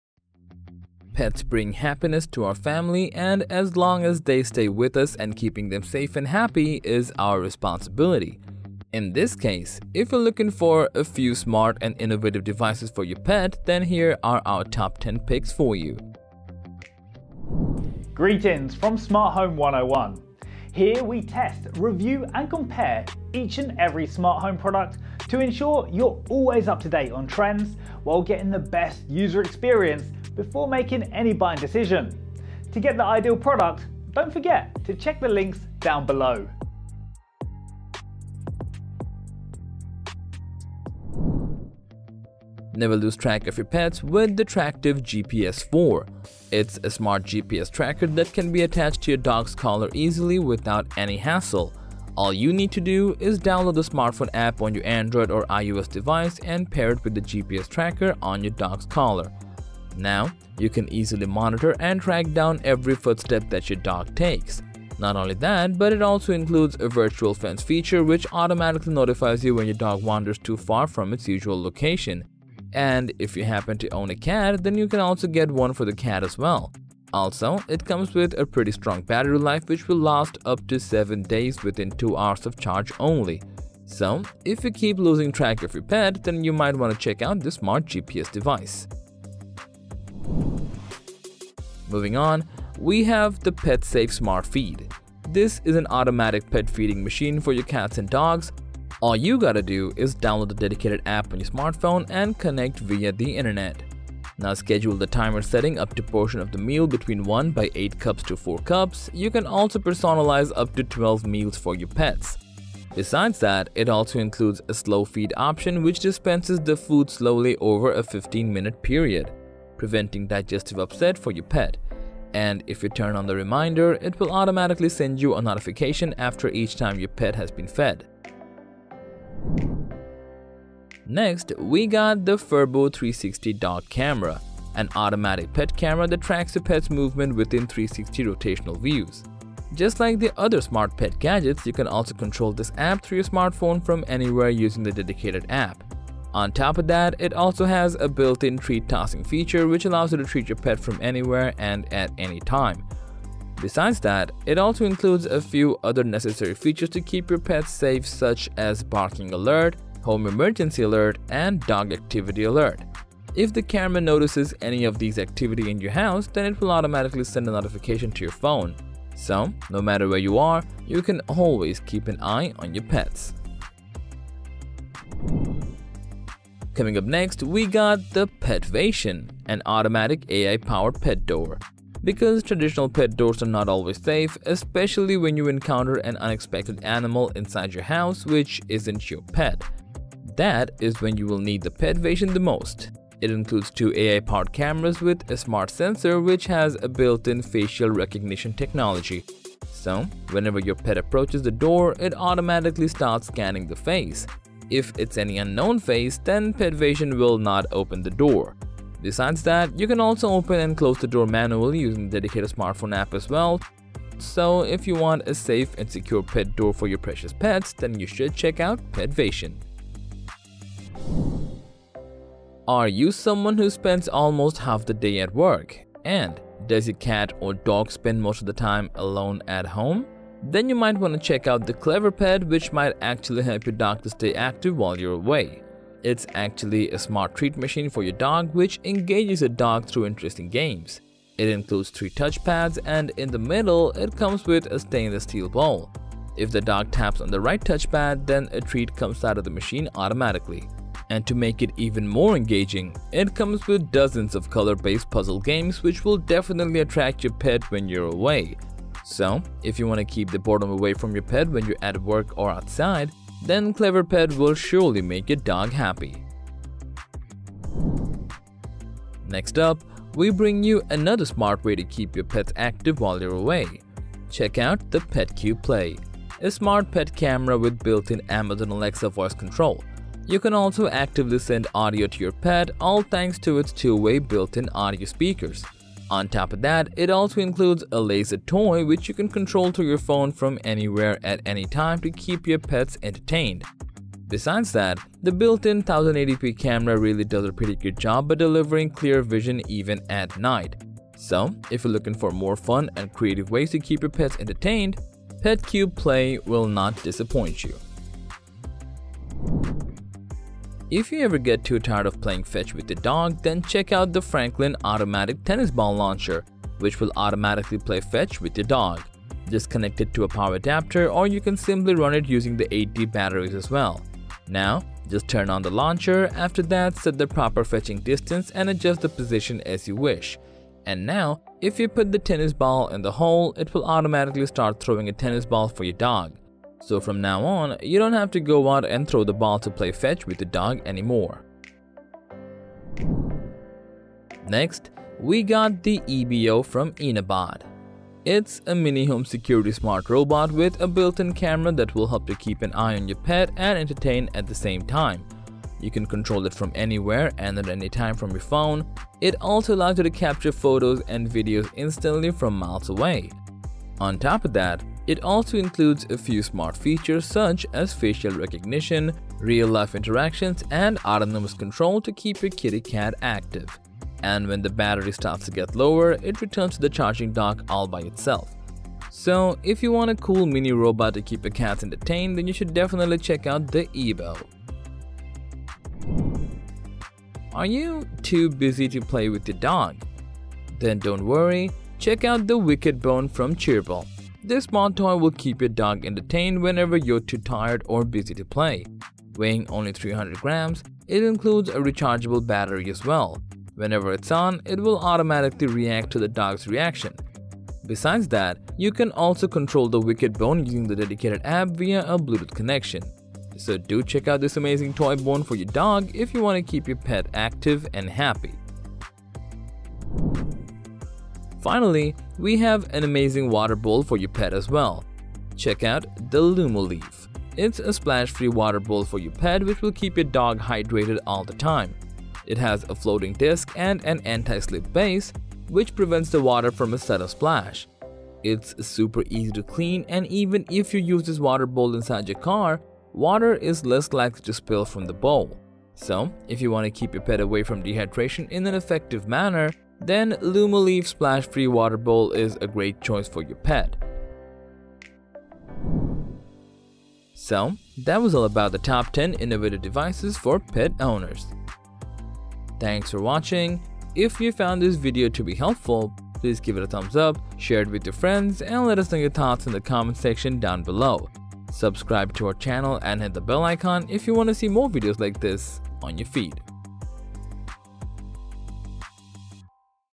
10 Innovative Pet Devices for Pet Owners!
Innovative pet devices have started to become more popular, as it keeps your pet safe and happy at the same time. So, if you are looking for some of the innovative pet devices on the market, then here are the top 10 picks for you.